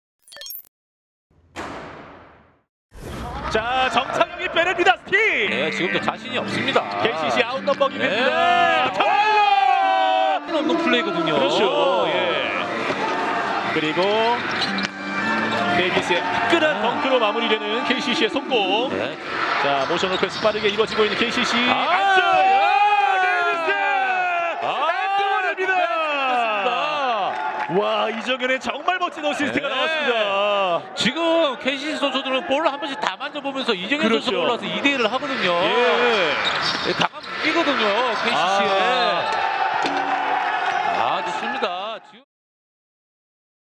kt 림 : 나 좀 살려줘요...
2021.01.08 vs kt

타일러 데이비스의 4Q 덩크 모음

함께 보시죠